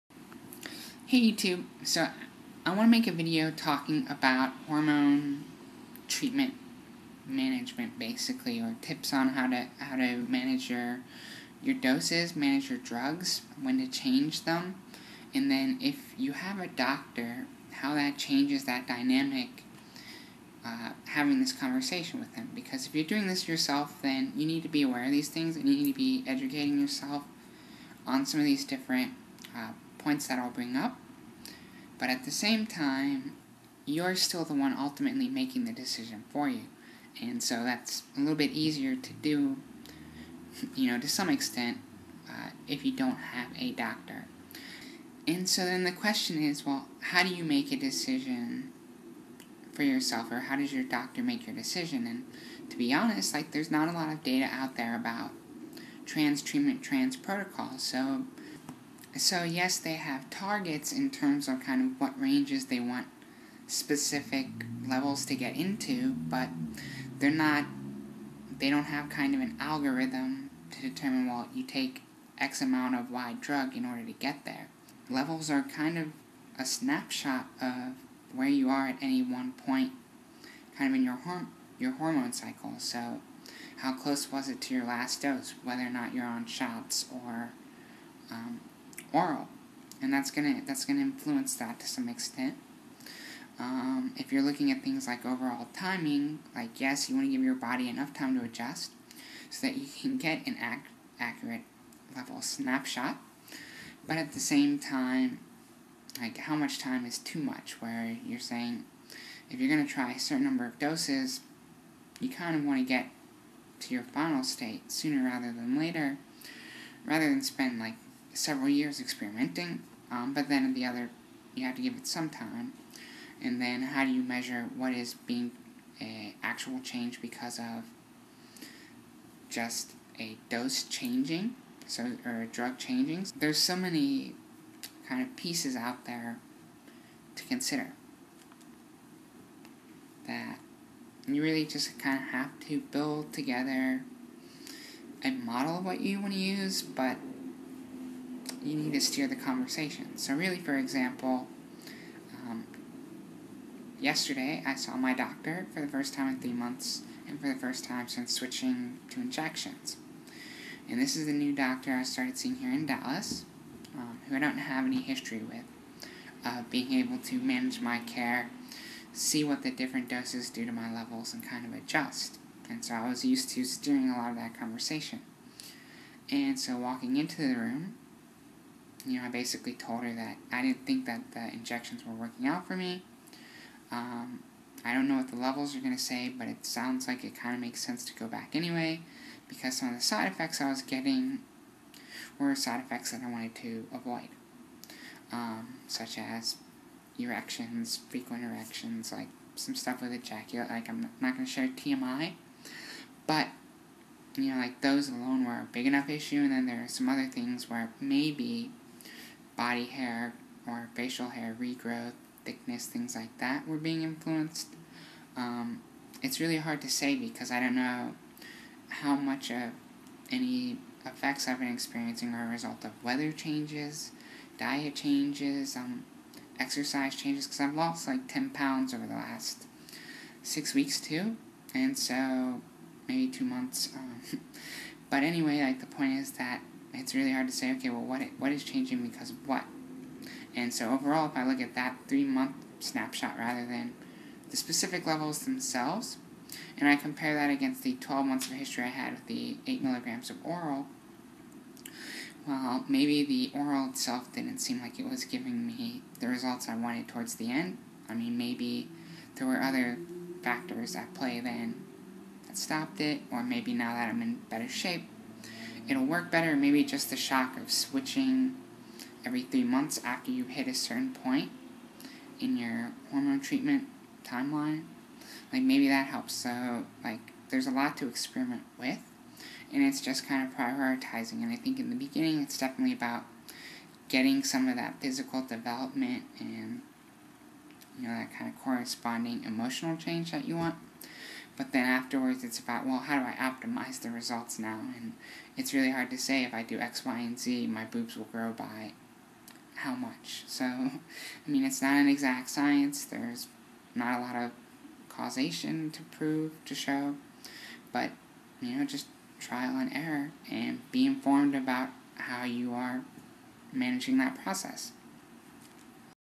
20unc06: HRT/hormone optimization for transitioning MTFs - when to change drugs and doses
I break down some of the different aspects to managing your hormone replacement therapy (HRT). While there are some levels that you should target (go look it up on Google, it's pretty easy to find), using levels, or 'timelines', certain physical changes, etc. is difficult because its hard to show causation in general, but especially because there are other external variables like diet, age, weight, metabolism, etc. that influence how you'd want to manage your own HRT accordingly. then you need to discuss your "plan" with a doctor if you're not doing it yourself, and what your doctor is familiar/comfortable with might not be the same as your plan. sooooooo.